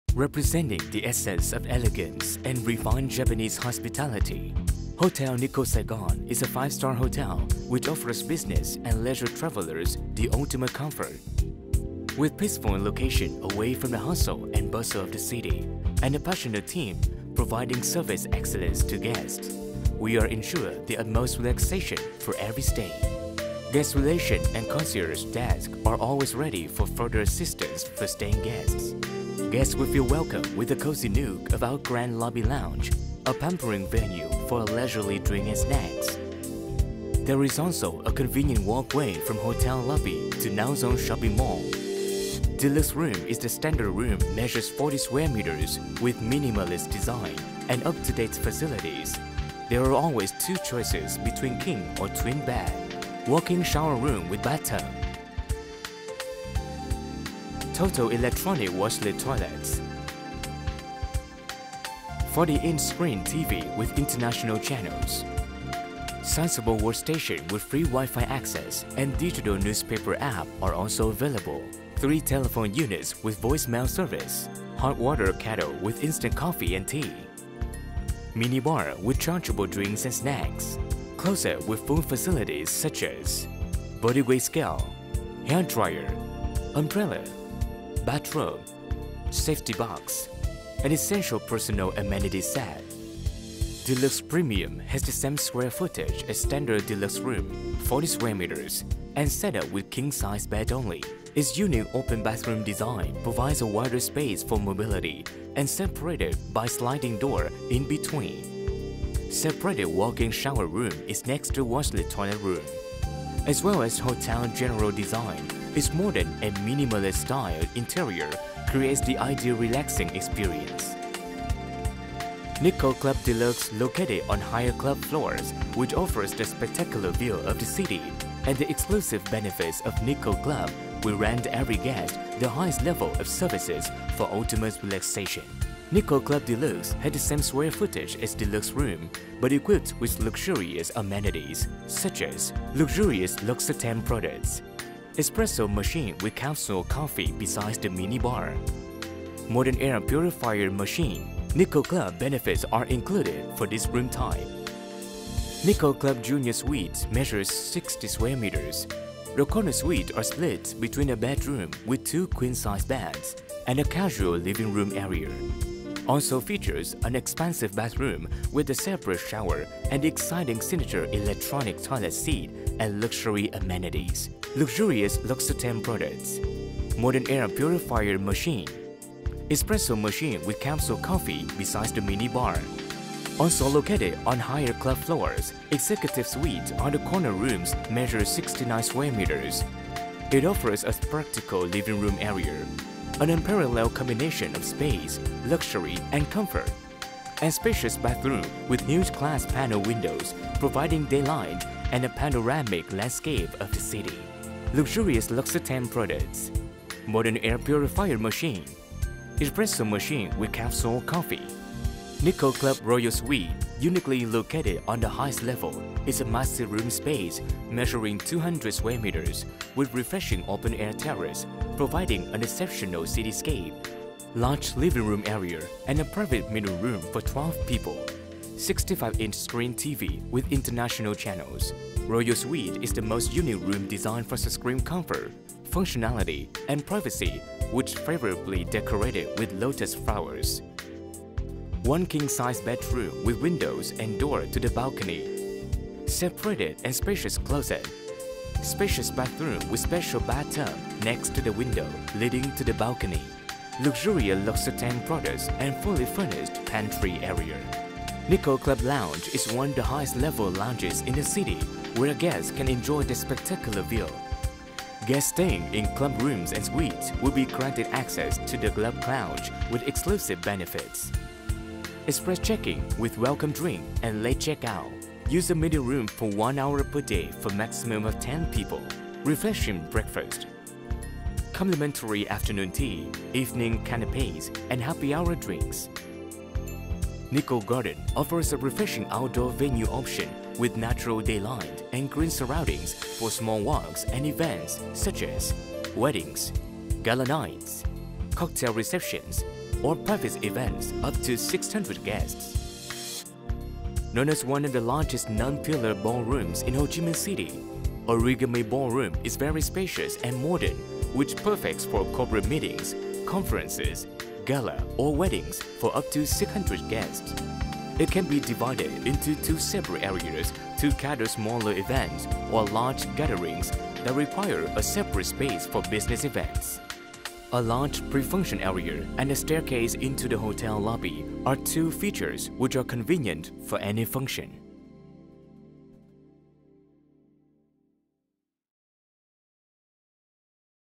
Hotel Nikko Saigon_Room Virtual Inspection (Viet Sub)
With a standard room size of 40sqm, our guest rooms are among the most spacious accommodation in the city.
Earth tone color theme room interior with minimalist design is a characteristic of Japanese that provides elegance and a truly relaxing stay experience.

Address: 235 Nguyen Van Cu Street, District 1, Ho Chi Minh City, Vietnam